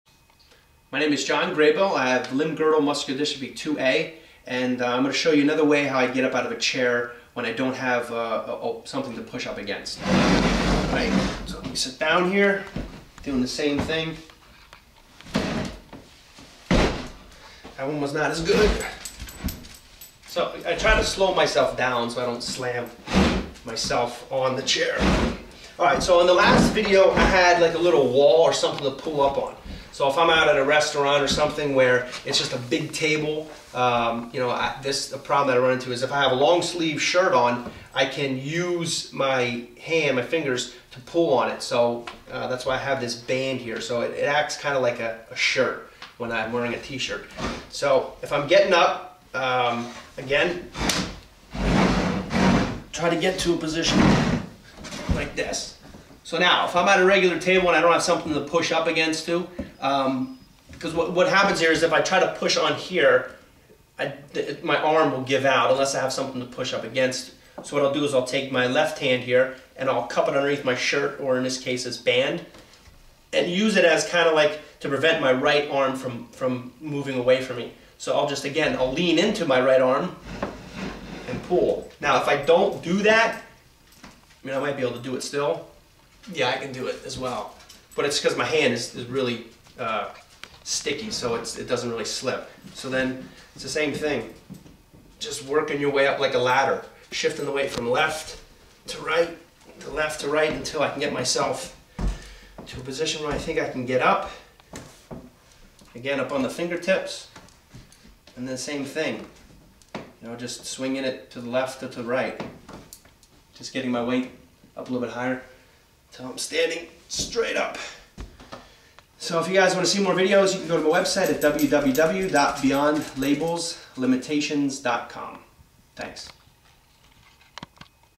LGMD2A: Rising from a Chair with a Wristband 3/08
Limb-Girdle Muscular Dystrophy 2A individual displaying the course of muscular dystrophy through the activity of rising from a chair with a wristband in March 2008.